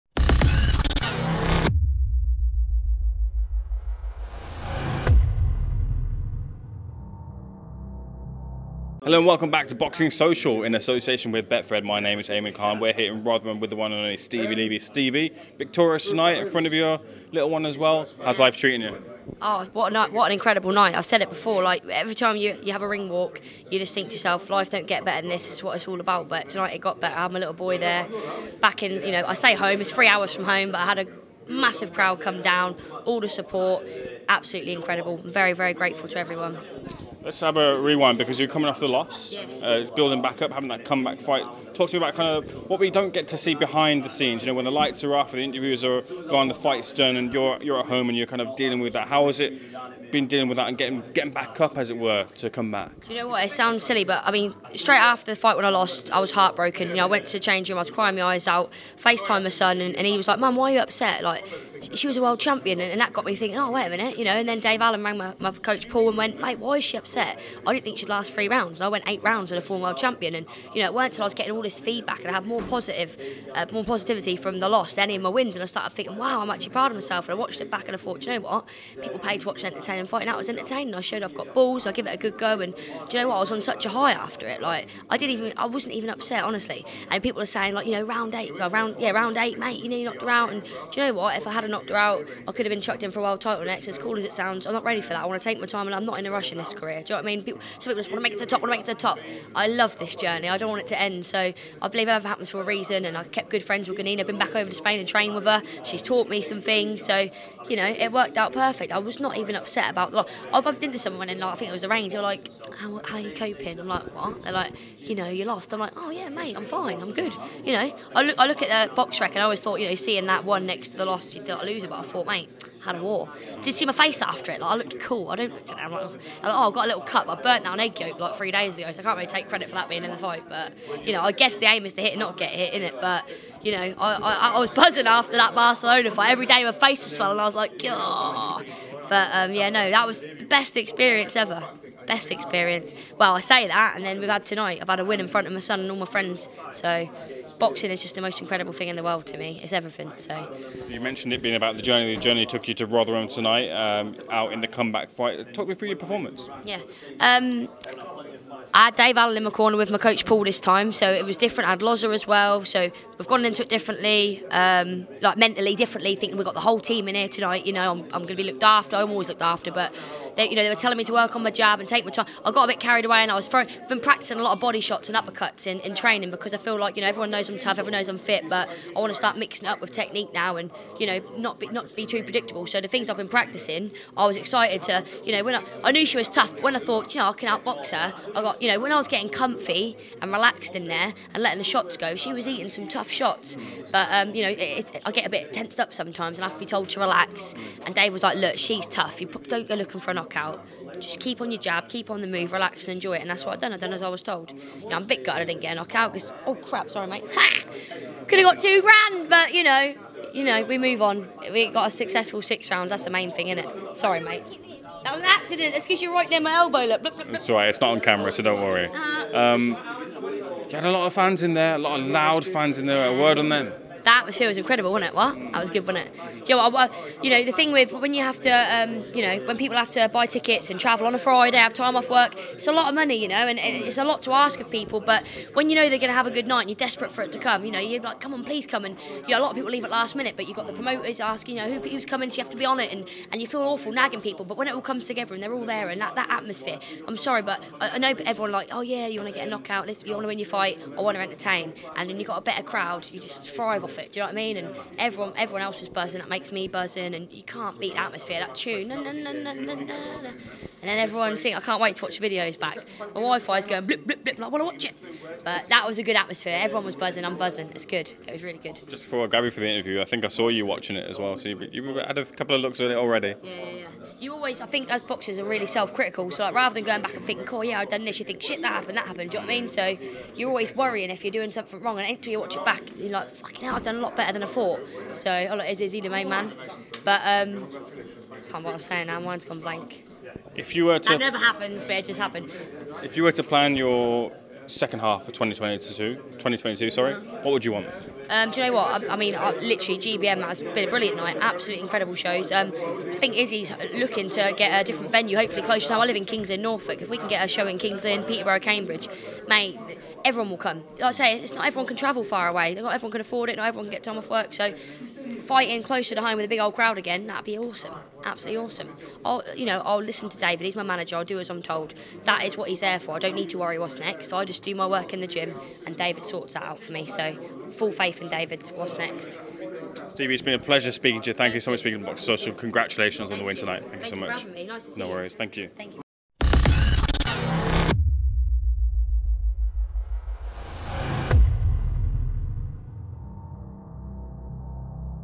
Stevi Levy Reacts To Comeback Points Win, Talks Guanini Defeat, Dave Allen Influence & More
Boxing Social's Ayman Khan spoke to Stevi Levy.